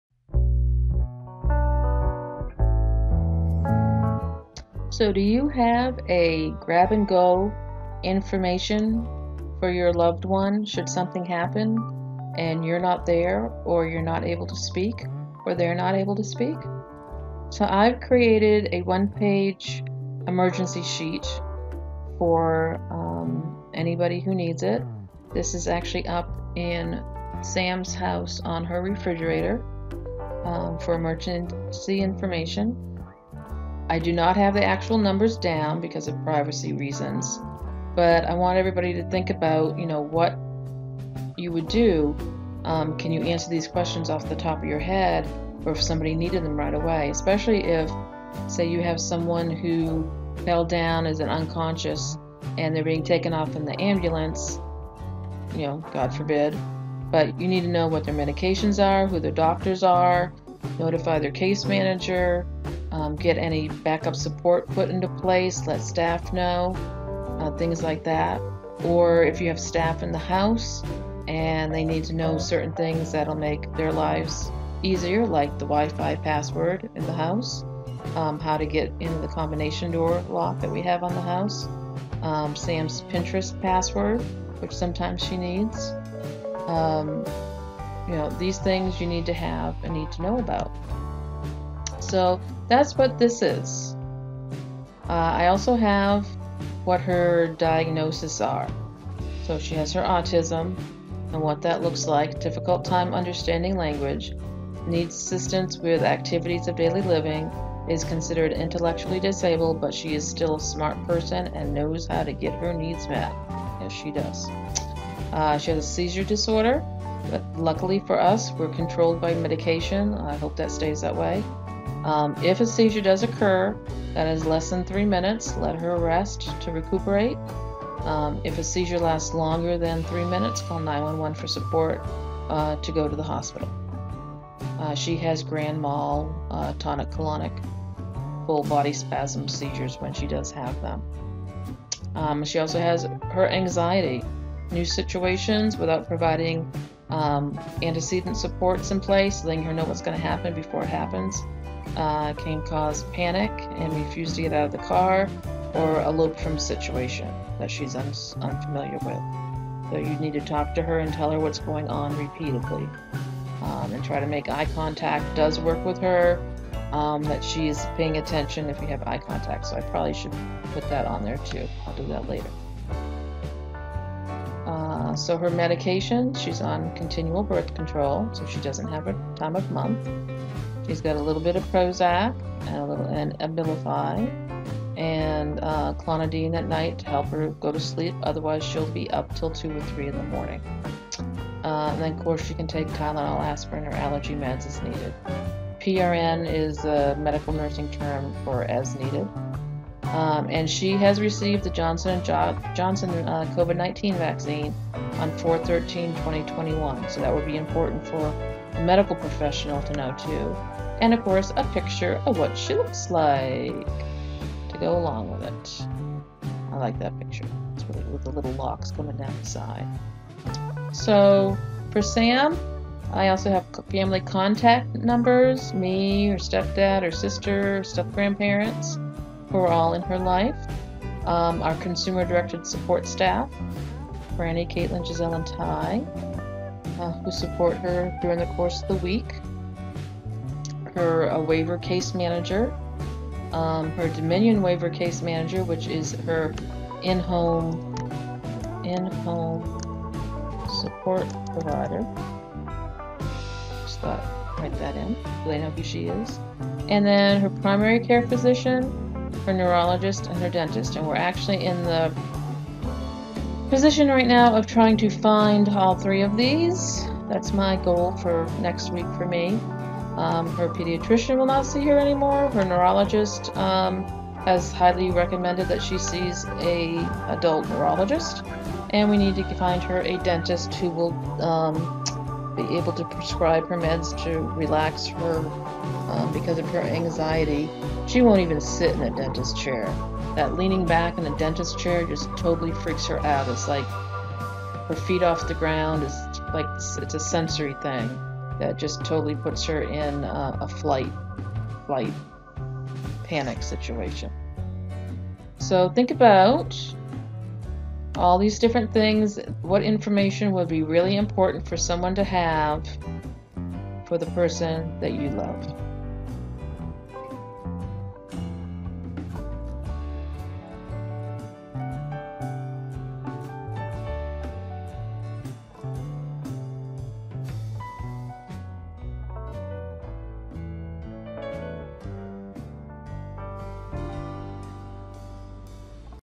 Emergency contact sheet for people who can't speak
If something happened to your loved one while in the care of others would they have all the information they need? If you were there would you be able to remember all the information you would need and all the people you would need to contact. The answer is creating a one page grab and go information emergency sheet you can put in a easily accessible location.
During this video I provide some other things you may not have thought about which make this piece of paper so important.